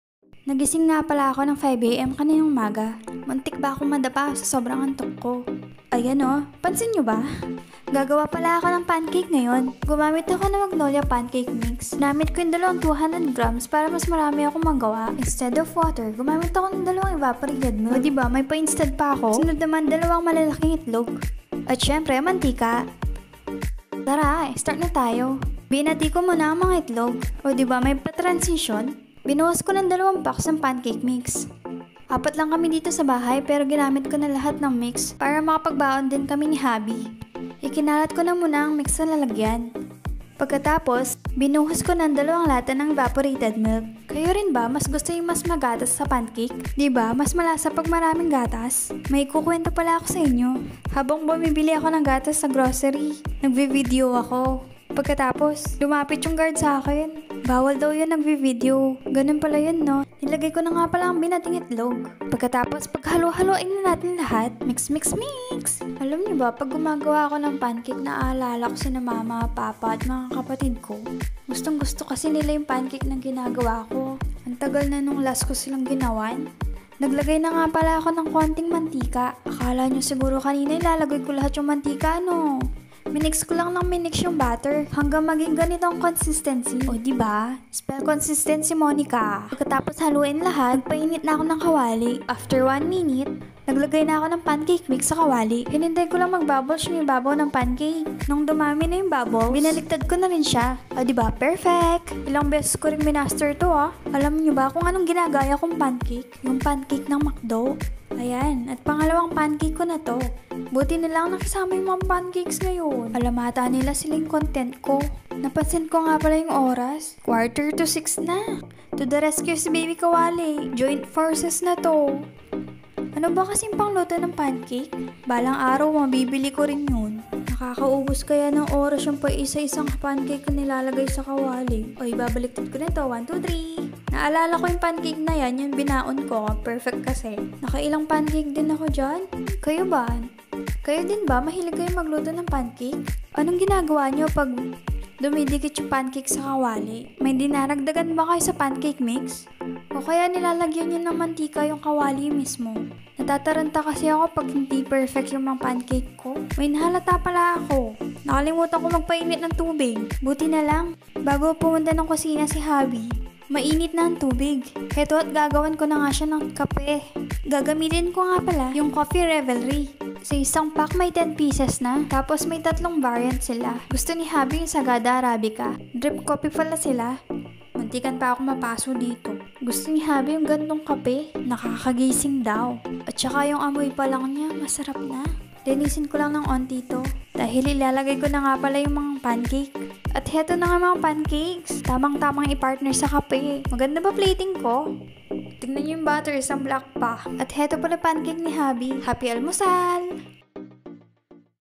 Gawa tayo pancakes para sa almusal 😋😋😋
Mahilig akong gumawa ng pancakes para sa family. Pag sawa na pritong itlog, ihalo na lang sa ibang pagkain kaya gumawa tayo ng pancakes for today's video.

Meron din akong recommended na coffee shop. Actually drip coffee sila. Meron po silang fb page at instagram account kung gusto nyong bumili.